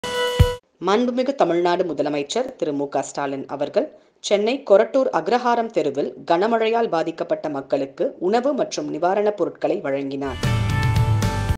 கொரட்டூர் அக்ரஹாரம் தெரு-கனமழையால் பாதிக்கப்பட்ட மக்களுக்கு உணவு மற்றும் நிவாரணபொருட்களை வழங்கினார்
மாண்புமிகு தமிழ்நாடு முதலமைச்சர் திரு.மு.க ஸ்டாலின் அவர்கள் சென்னை கொரட்டூர் அக்ரஹாரம் தெருவில் கனமழையால் பாதிக்கப்பட்ட மக்களுக்கு உணவு மற்றும் நிவாரண பொருட்களை வழங்கினார்